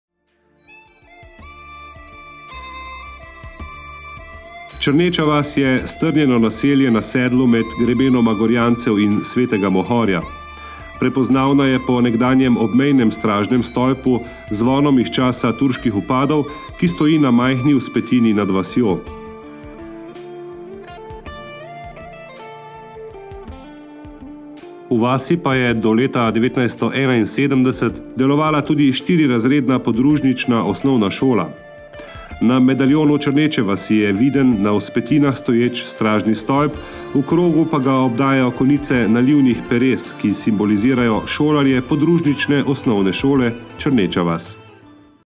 Črneča vas (Insignija Občine Kostanjevica na Krki)
Črneča vas je strnjeno naselje na sedlu med grebenoma Gorjancev in svetega Mohorja. Prepoznavna je po nekdanjem obmejnem stražnem stolpu z zvonom iz časa turških vpadov, ki stoji na majhni vzpetini nad vasjo. V njej pa je do leta 1971 delovala tudi štirirazredna podružnična osnovna šola. Na insigniji Črneče vasi je viden na vzpetinah stoječ stražni stolp, v krogu pa ga obdajajo konice nalivnih peres, ki simbolizirajo šolarje podružnične osnovne šole Črneča vas.

Snemalec in montažer: Boštjan Colarič
Besedilo: Anita Grubar in Janja Baznik
Pomoč pri snemanju: Matjaž Krhin

Županovo verigo občine Kostanjevica na Krki sta oblikovali Eva Seljan in Janja Baznik.
Županovo verigo so izdelali v Zlatarstvu Koman.